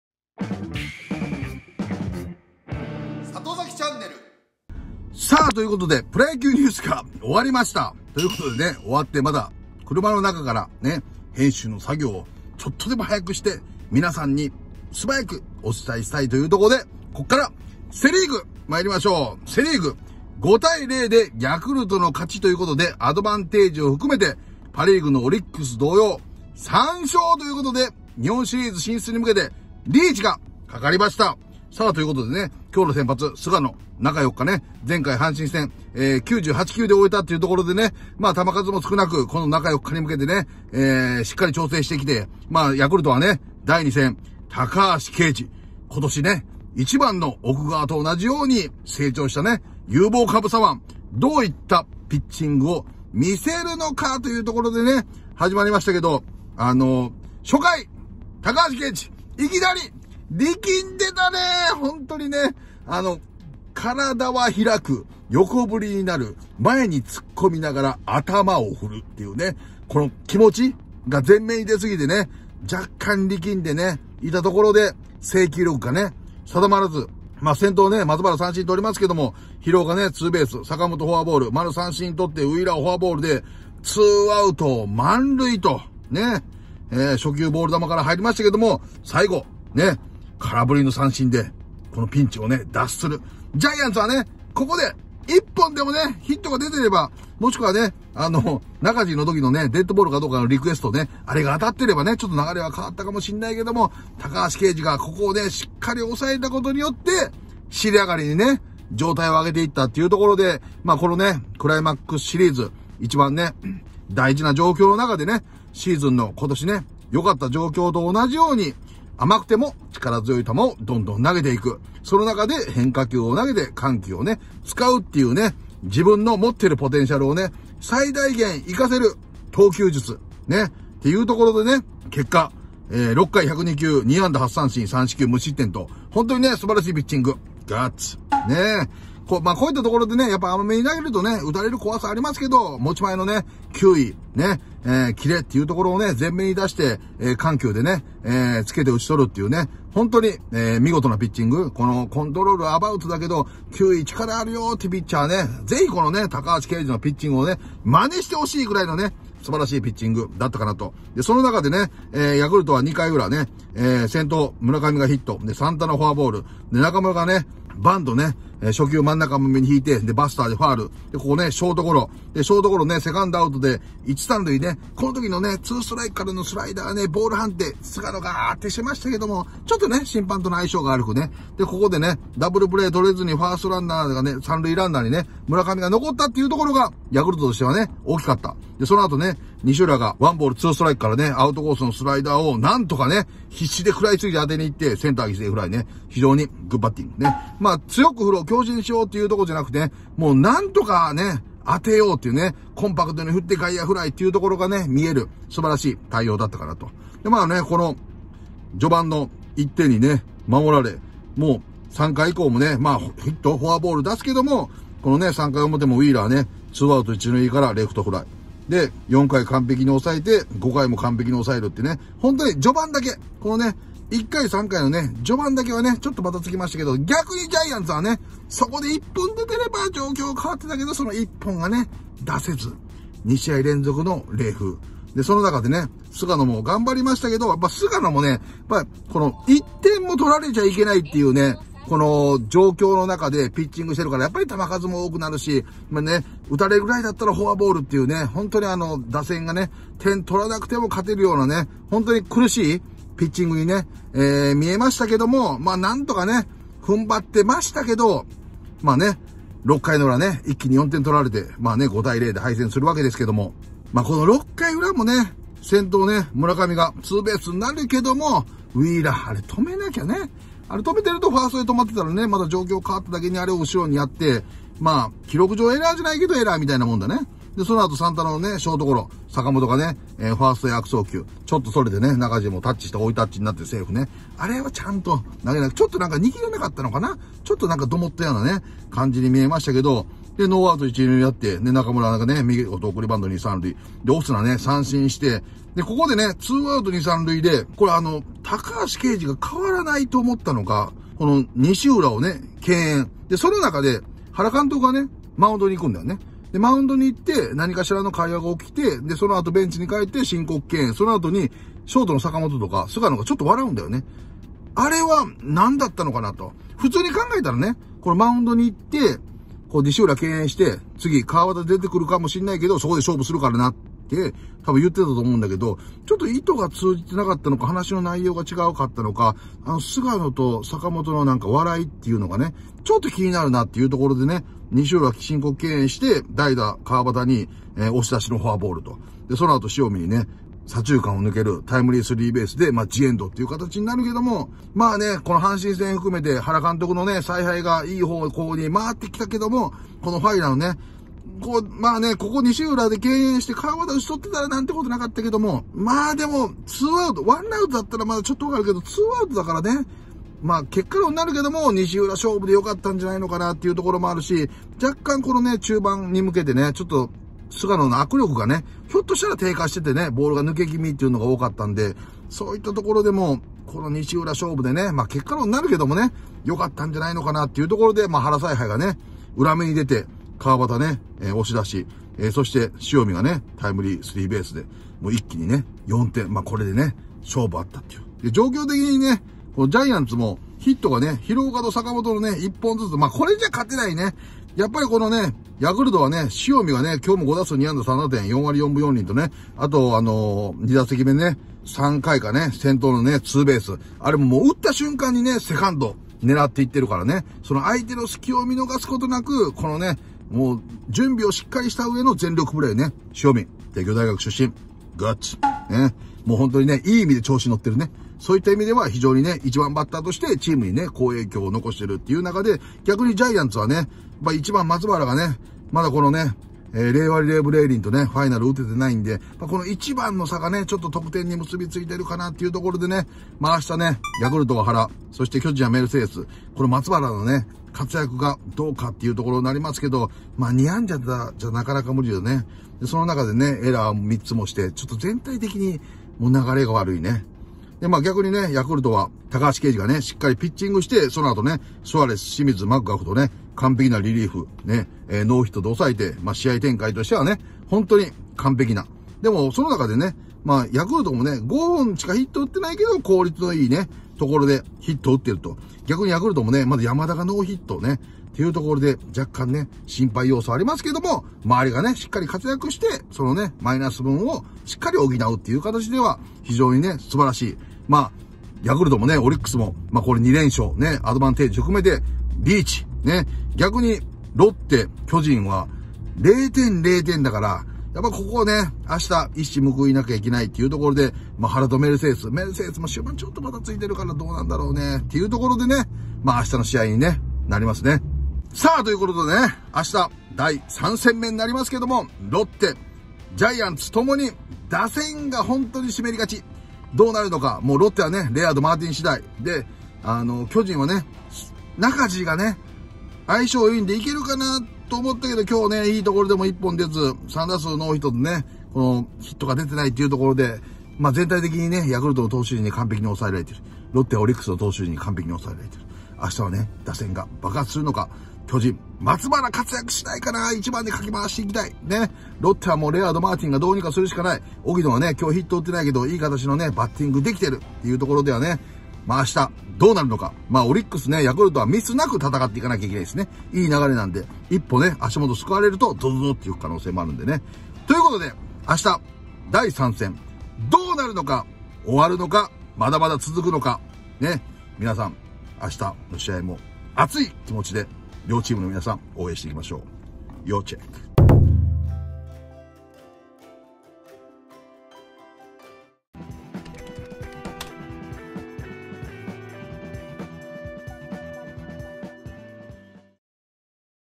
高橋奎二が6回無失点でヤクルトが2連勝！巨人は第3戦この選手がキーマンになる！この試合のGOOD・BADプレーをどこよりも詳しく解説します【プロ野球ニュース】【プロ野球】
2006WBC優勝！ベストナイン！ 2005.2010ロッテ日本一！